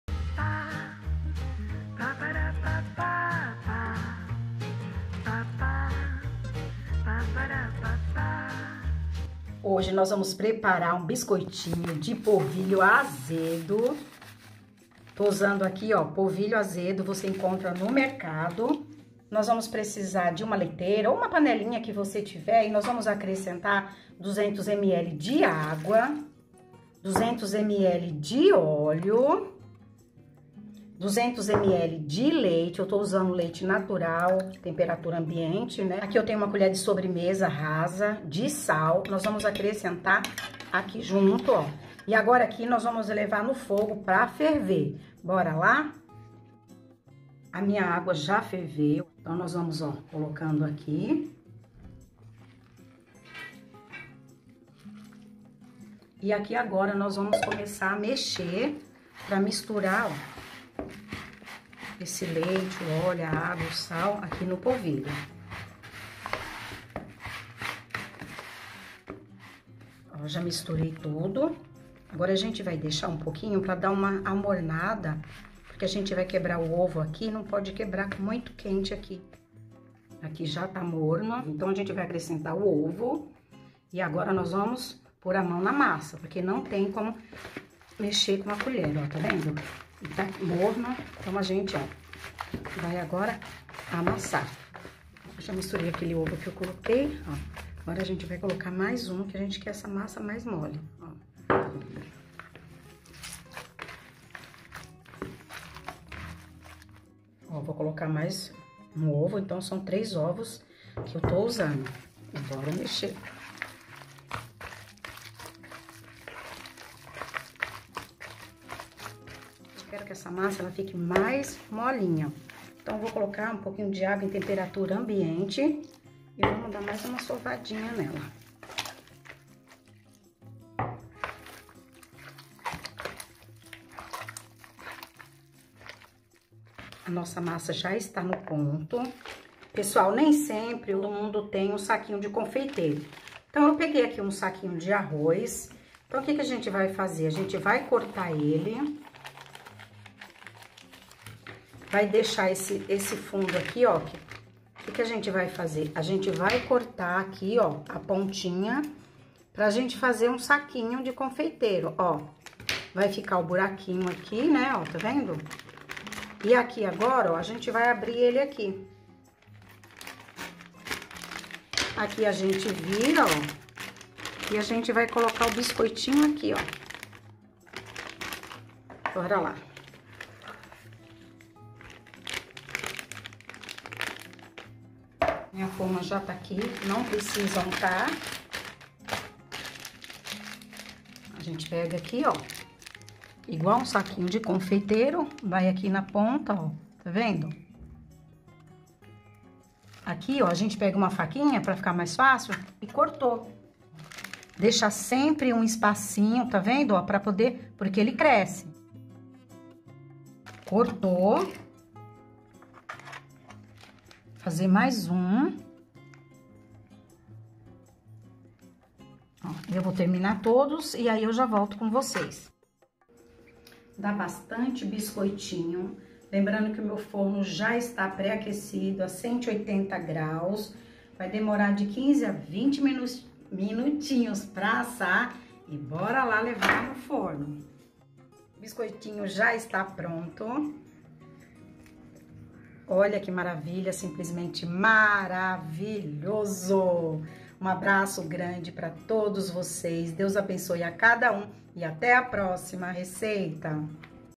✅️ COMO FAZER BISCOITO ASSADO NO FORNO DELICIOSO PARA O CAFÉ DA TARDE
Hora do café pede um biscoito de polvilho delicioso! Receita simples e fácil de fazer
A família inteira vai amar
Eu me chamo Dinda e vou compartilhar essa receita deliciosa
ingredientes
500g de polvilho
minha xícara é de 200ml
1 xícara de água
1 xícara de óleo
1 xícara de leite
1 colher sobremesa rasa de sal
3 ovos
Confira como fazer biscoito de polvilho azedo no passo a passo no vídeo
Deus abençoe